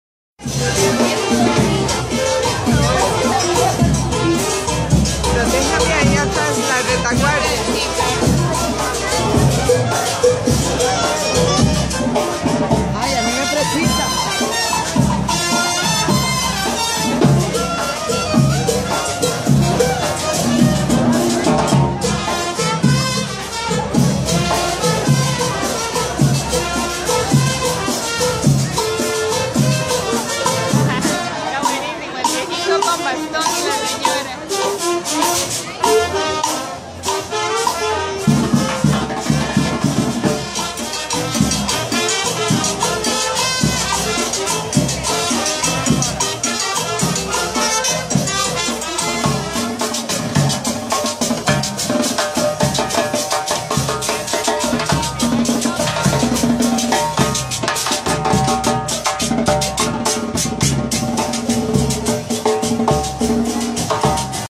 Payasos en la avenida central de San José
Los payasos bailan en Navidad